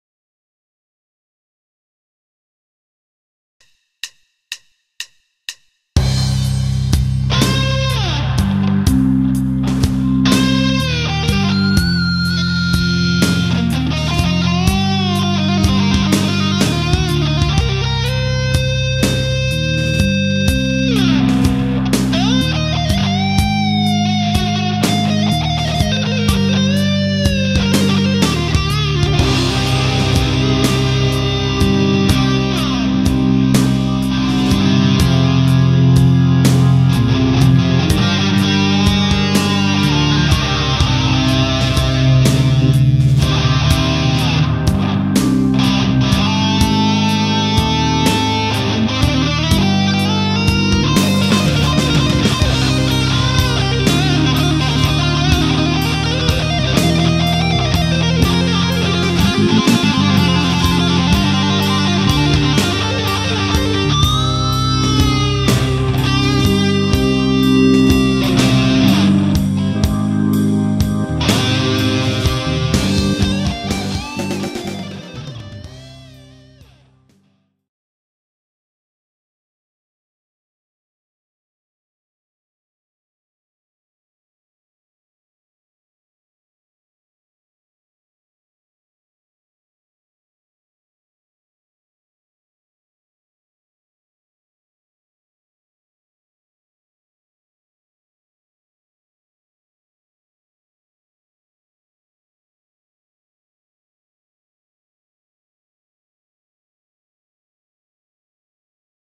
Blues Guitar Solo
Me just playing over a blues backing track. Its completely  improvised in C Minor. This was played on a Les Paul though a JCM 800 2210. Ive had a few problems with my Guv'nor distortion pedal which i use and had to resort in using another pedal, hence it doesnt sound that great.